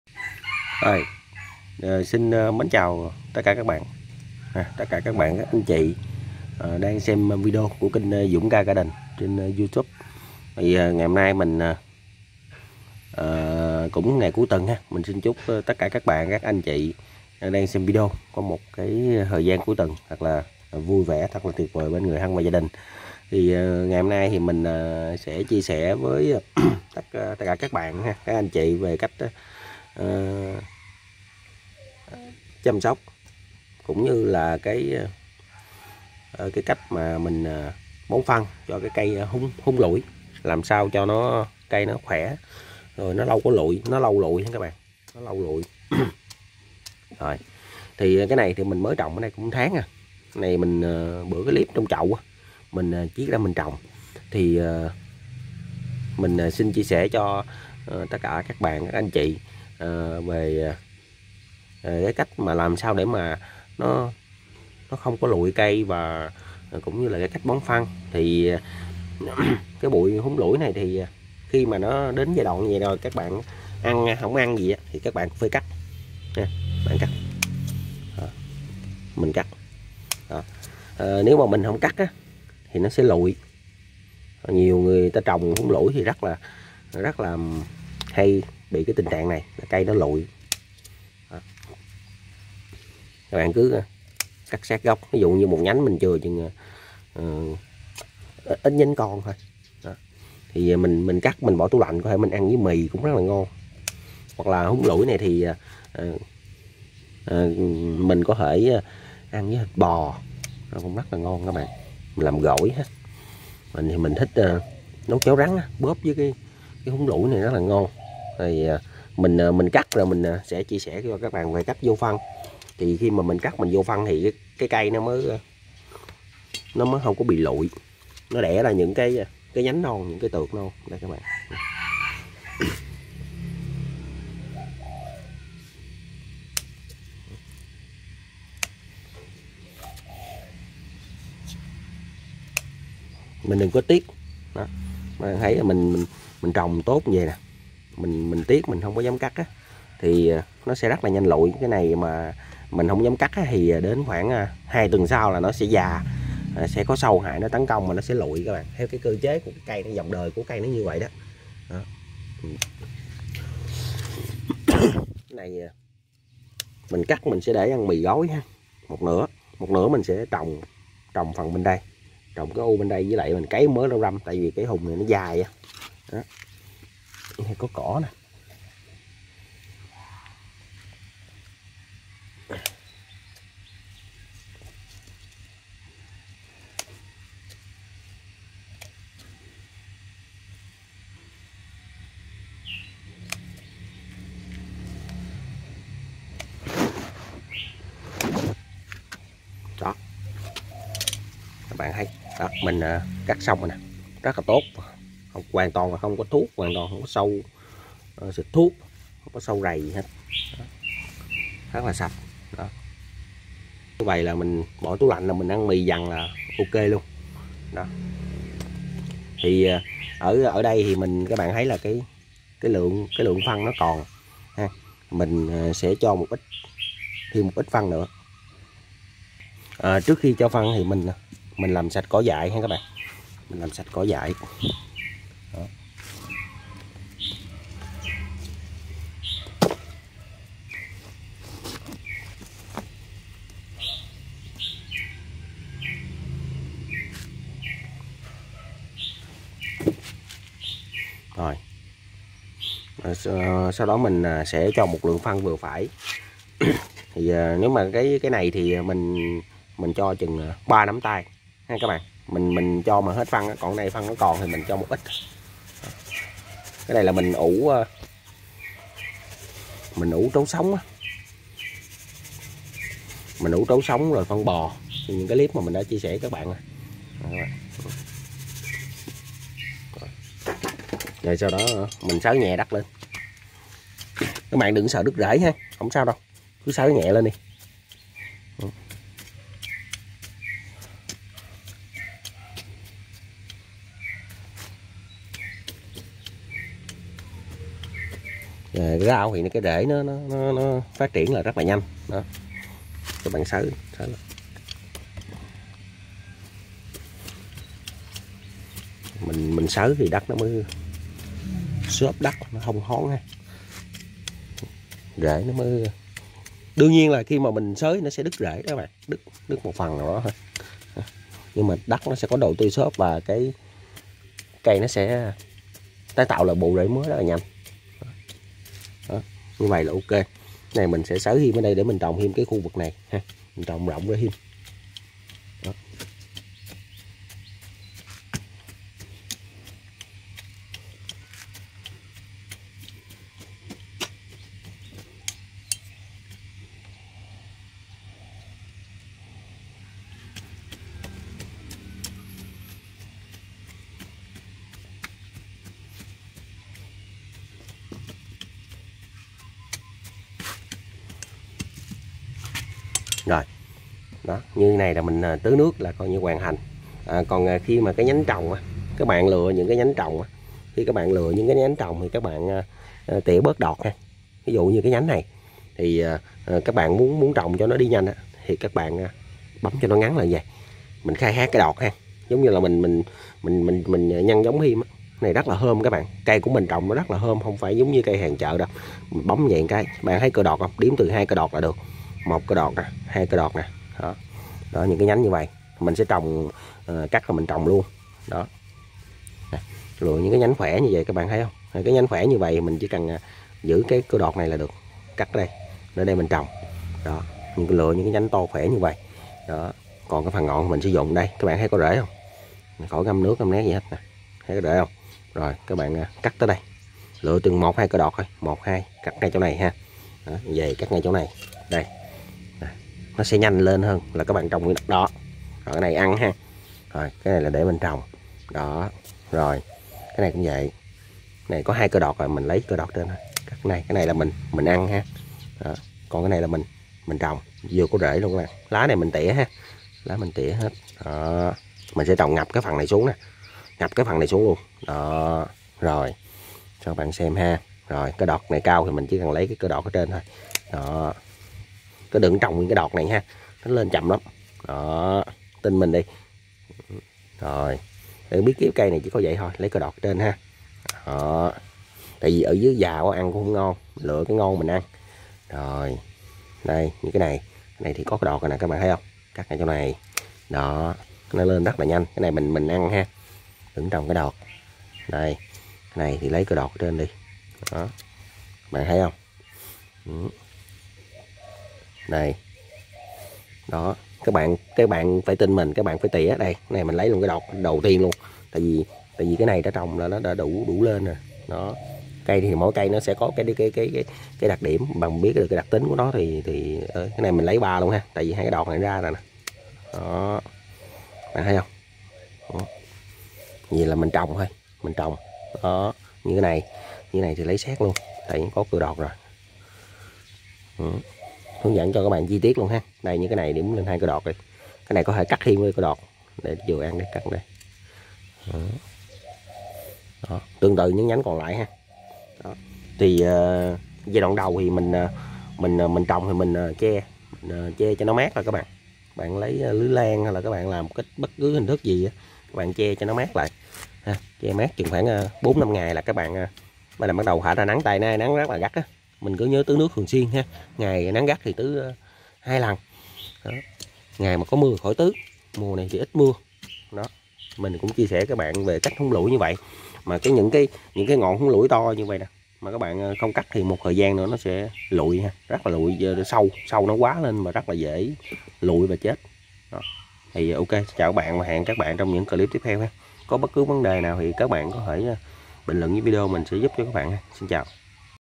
Cách Trồng Cách Chăm Sóc Cách Bón Phân Rau Húng Lủi Khỏe Lâu Tàn
Dũng Ka GarDen chia sẽ: Cách Trồng Cách Chăm Sóc Cách Bón Phân Rau Húng Lủi Khỏe Lâu Tàn